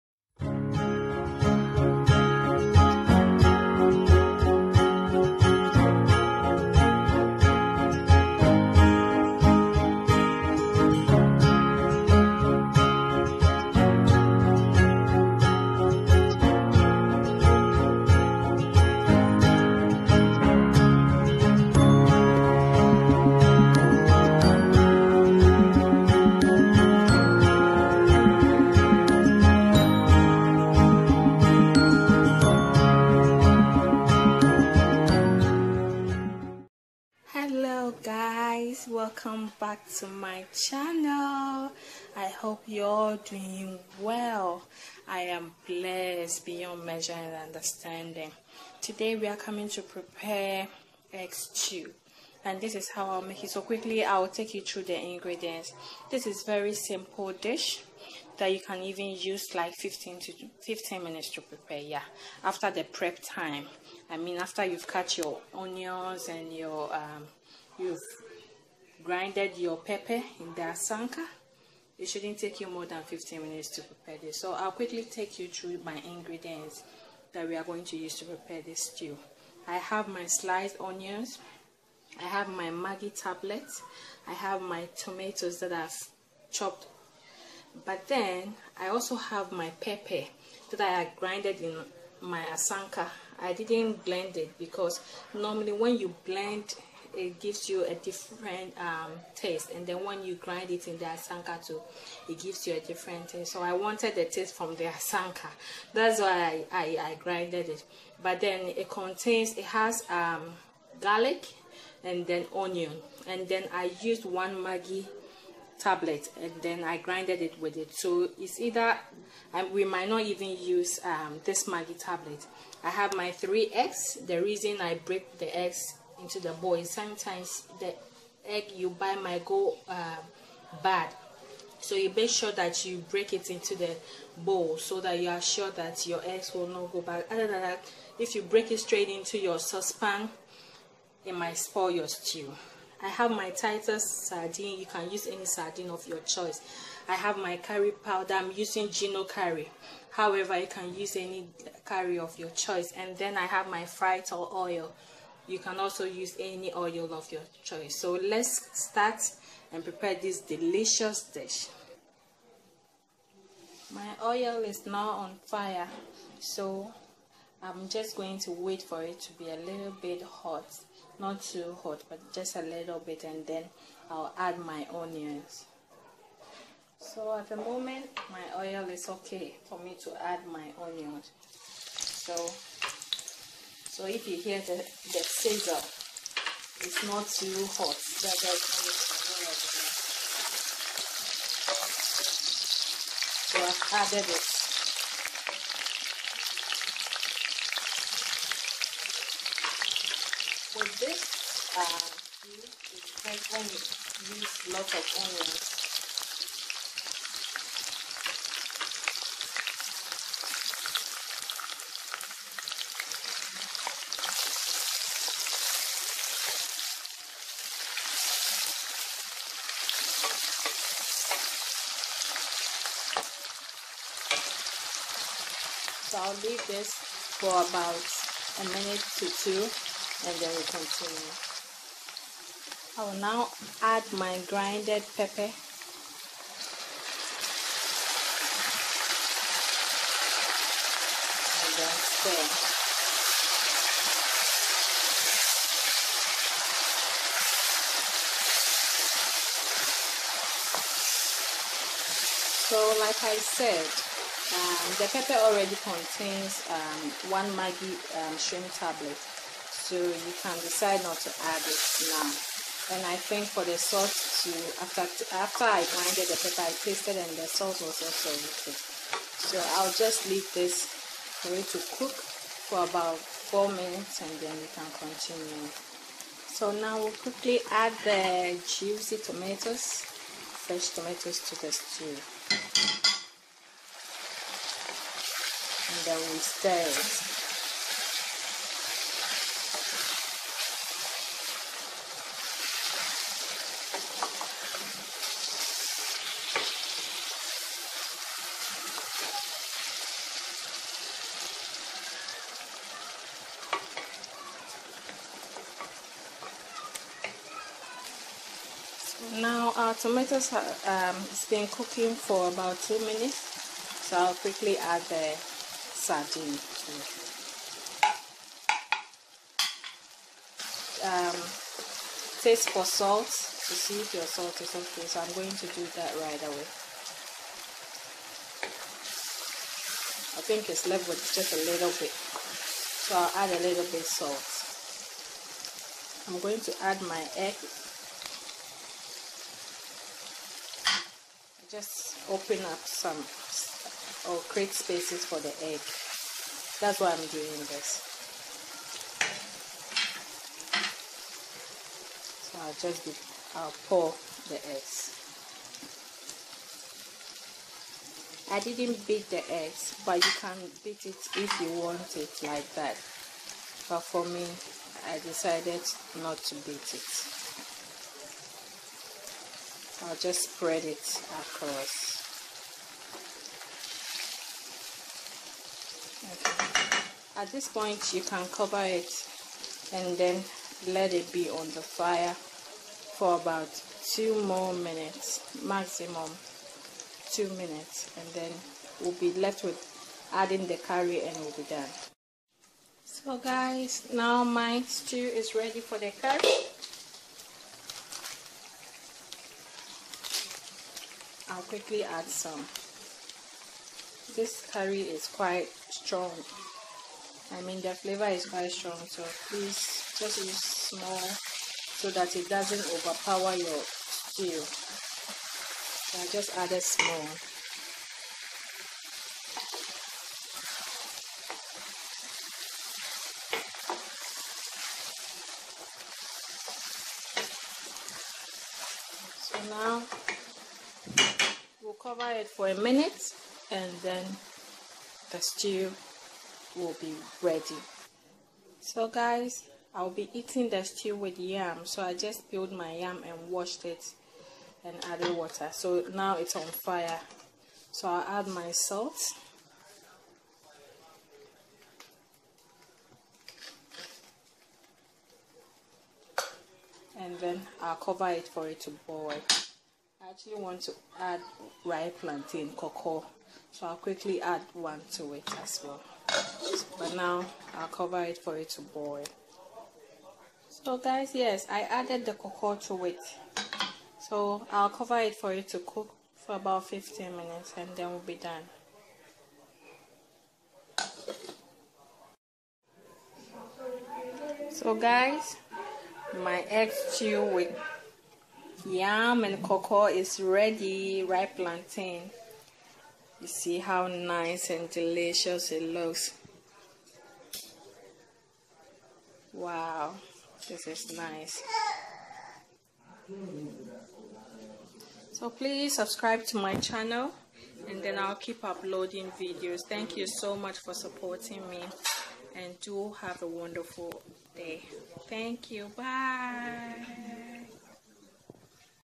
DELICIOUS EGG STEW MADE SIMPLE!!!
Ingredients
Oil
Onions
Pepper
Garlic
Maggie shrimp tablet
Fresh tomatoes
Sardine
Curry powder